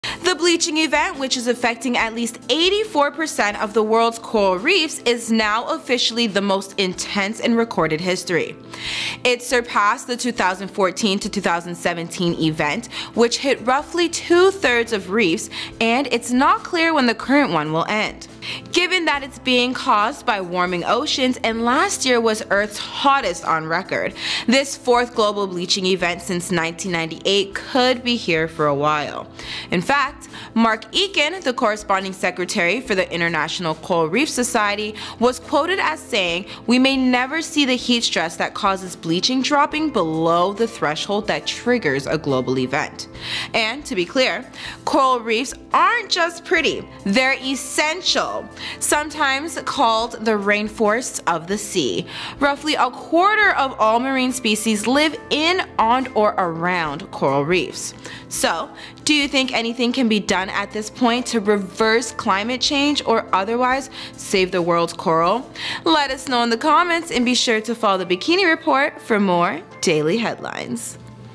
THE CORAL REEFS BLEACHING EVENT IS NOW THE MOST INTENSE IN RECORDED HISTORY! Tia LaRose
The bleaching event which is affecting at least EIGHTY-FOUR PERCENT of the world's coral reefs is now officially the most intense in recorded history!

It's surpassed the 2014-to-2017 event which hit roughly two-thirds of reefs. and it's not clear when the current one will end!

Given that it's being caused by warming oceans, and last year was Earth's hottest on record, this fourth global bleaching event since 1998 could be here for a while!

In fact, Mark Eakin, the corresponding secretary for the International Coral Reef Society, was quoted as saying, ""We may never see the heat stress that causes bleaching dropping below the threshold that triggers a global event."

And to be clear, coral reefs aren't just pretty; they're essential! Sometimes called "the rainforests of the sea", roughly a QUARTER of all marine species live in, on, or around coral reefs!

So, do you think anything can be done at this point to reverse climate change or otherwise save the world's coral?

Powered by Naked News!